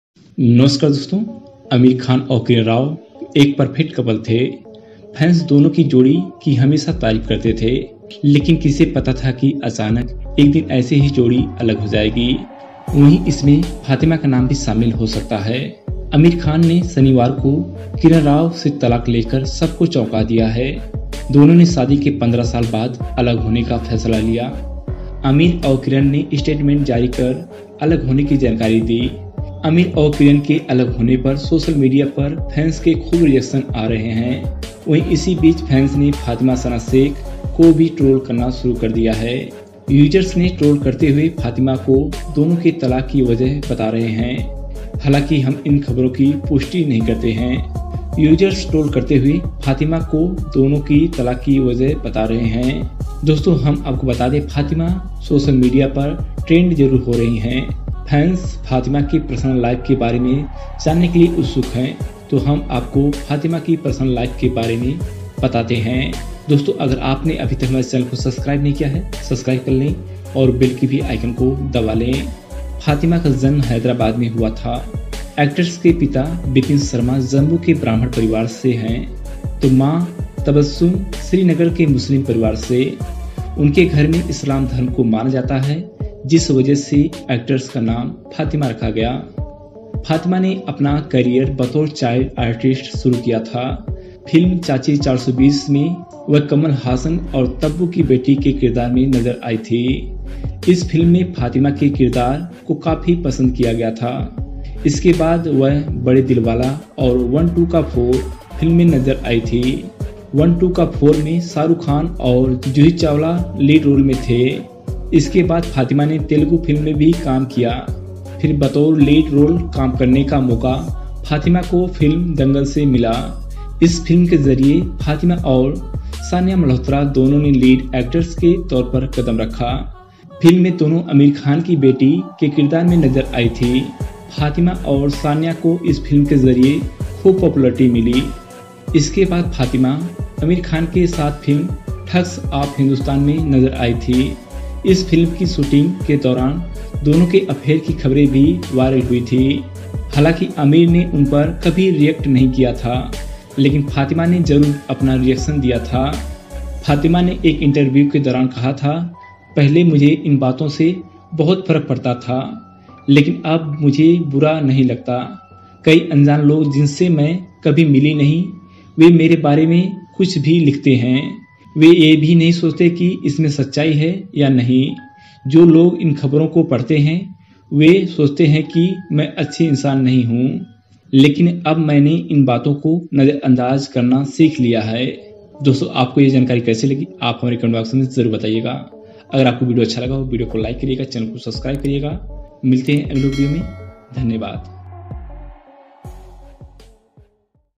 फातिमा सना शेख हैं ब्राह्मण परिवार से, आमिर खान के साथ नाम जुड़ने पर दिया था ये बड़ा बयान
Fatima sana Shaikh
 Amir khan
Aamir Khan Divorce